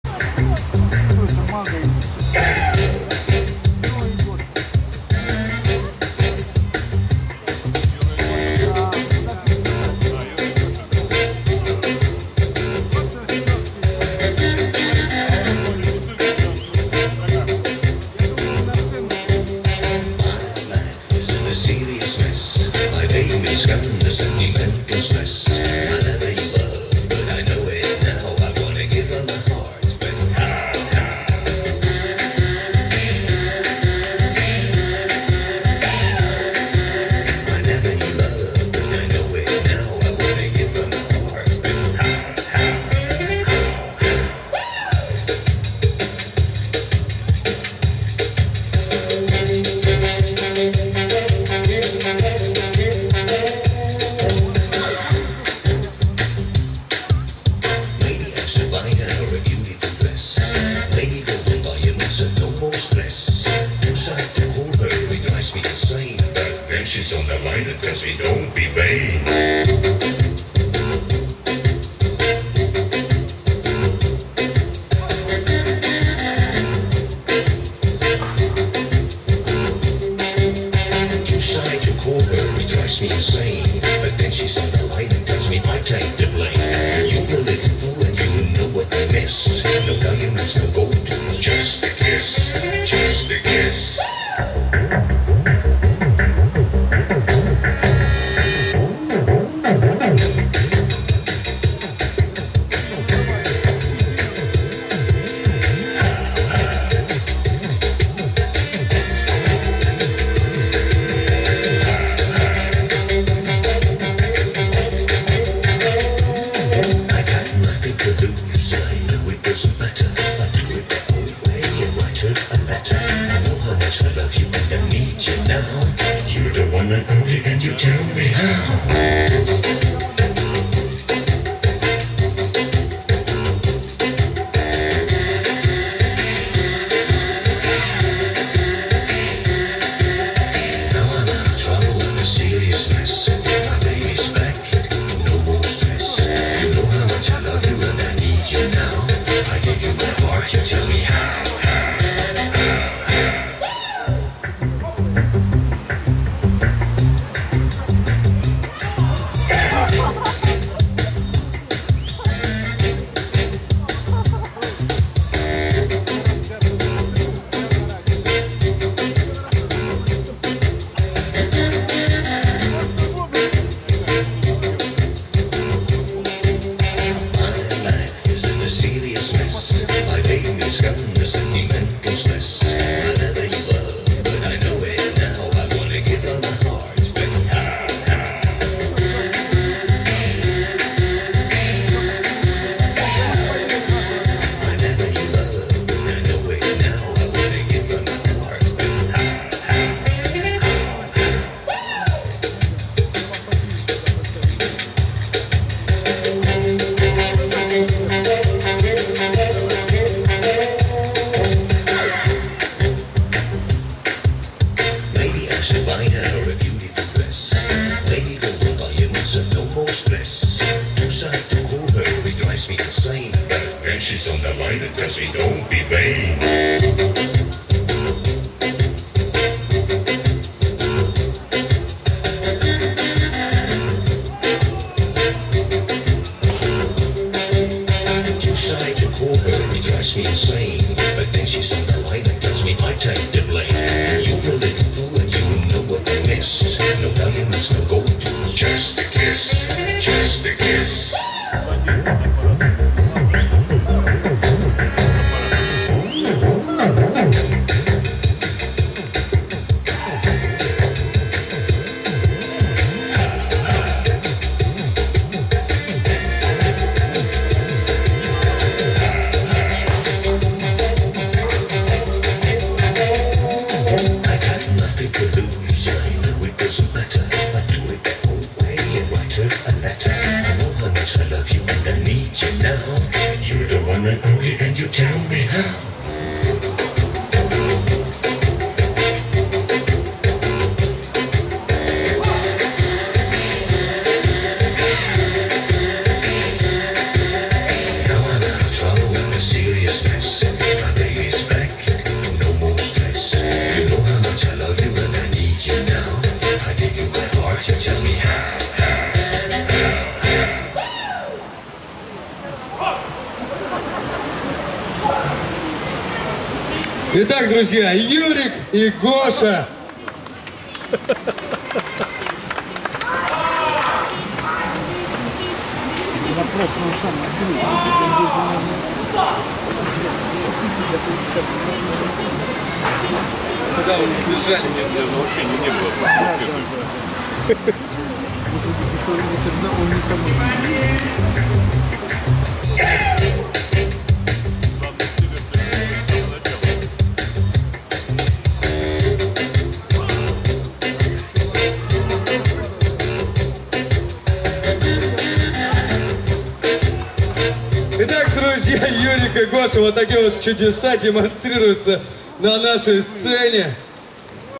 Клоуны Юрик и Гоша (2011)
Клоунский дуэт «Юрик и Гоша» - Юрий Бирюков и Георгий Дмитриев (Москва)

Второй Общедоступный Международный Фестиваль Клоунского Искусства «Звездопад клоунады в Сокольниках - 2011».

Главная сцена парка "Сокольники" 10 сентября 2011 года.